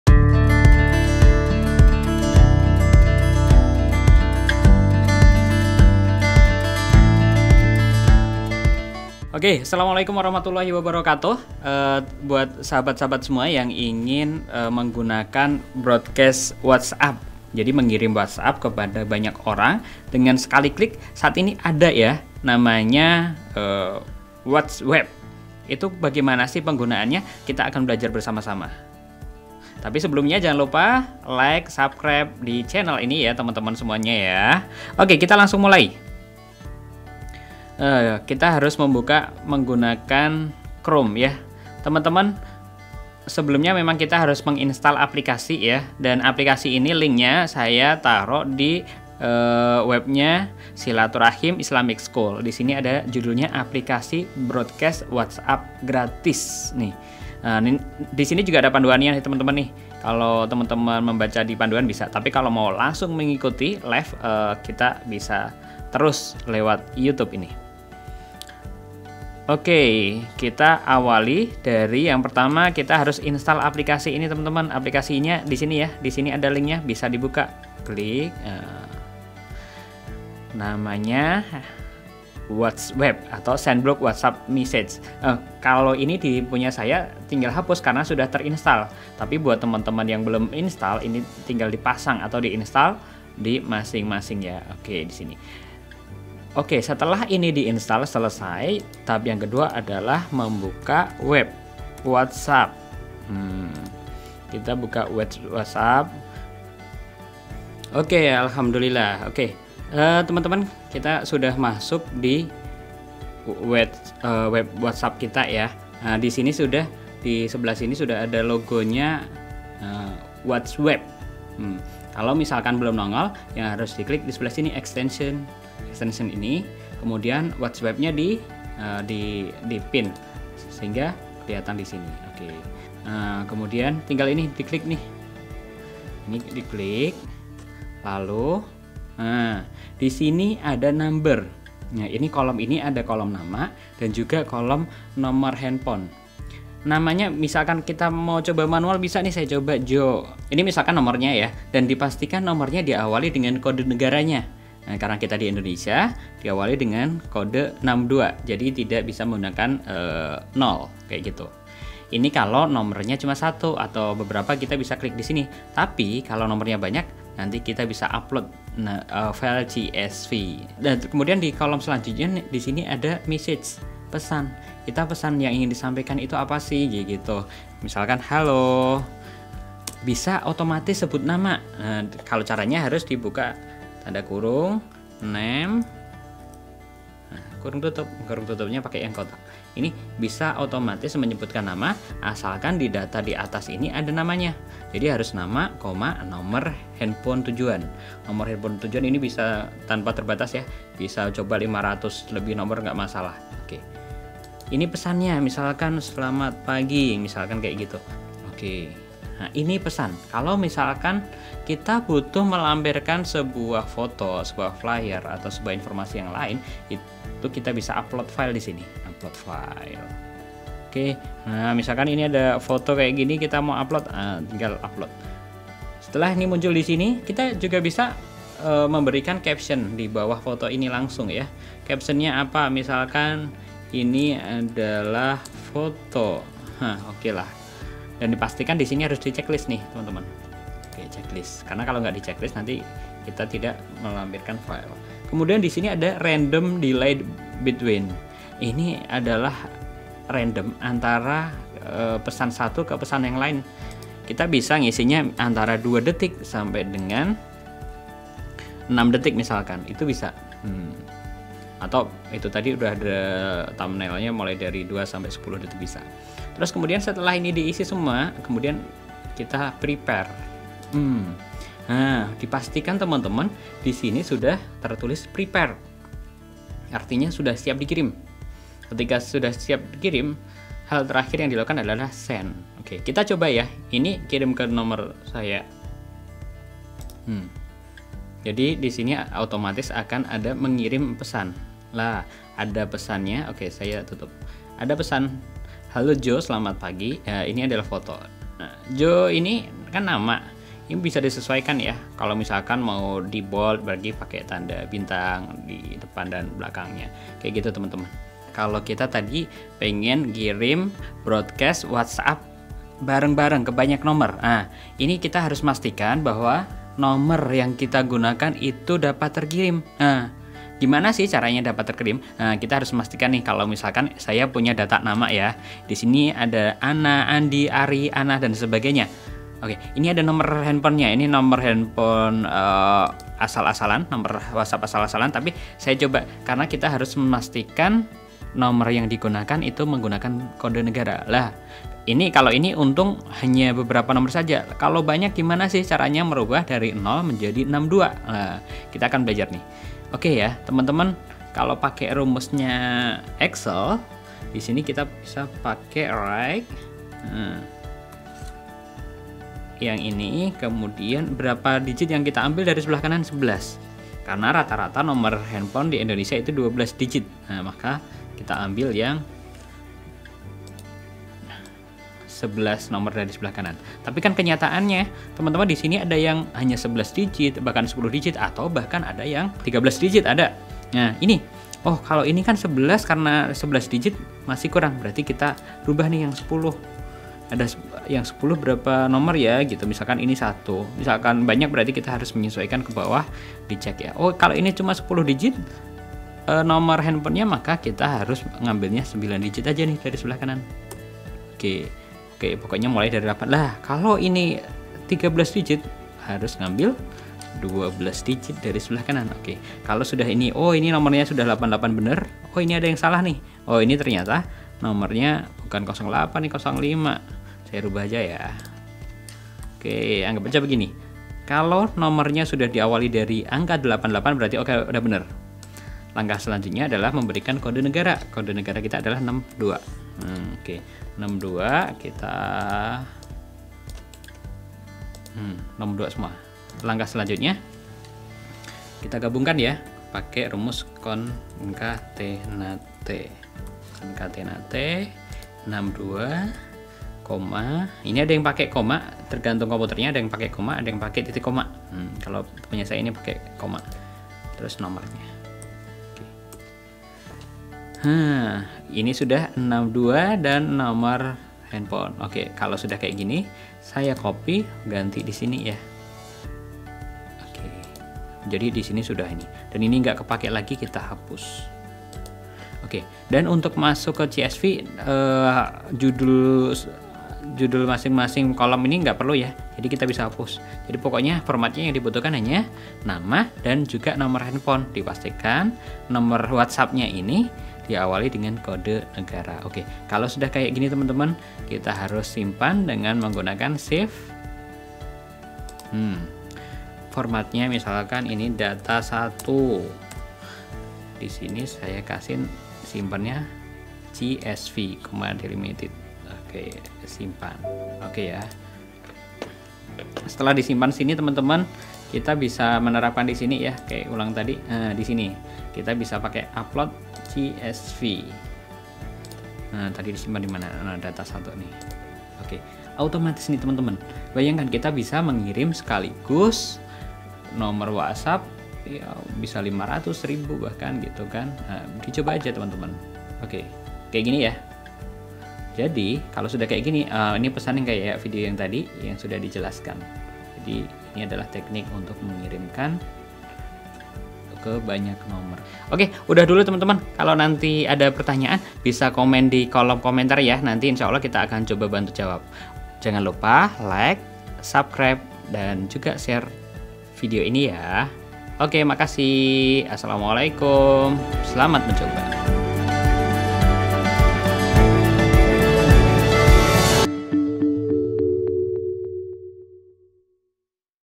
APLIKASI BROADCAST WHATSAPP GRATIS - MAS JOE
Temen-temen untuk Tutorial Terbaru bisa buka di link ini ya karena aplikasi ini ternyata mengalami perubahan. Terima kasih

Aplikasi Broadcast Whatsapp Gratis, ya bener gratis menggunakan aplikasi ini. Eh lebih tepatnya ini bukan aplikasi hanya sekedar pengaya atau add on WhatsWeb di Google Chrome.

Kamu dapat mengirim pesan whatsapp ke nomor banyak orang sekaligus.
Kamu hemat banyak waktu karena bisa mengirim ke nomor orang yang belum disimpan sekalipun.
Kamu bisa juga sebut nama orang yang kamu tuju secara otomatis tanpa harus edit satu persatu.
Yang jelas aplikasi ini gratis ya tanpa perlu beli yang macem-macem
Oke langsung saja pelajari cara penggunaannya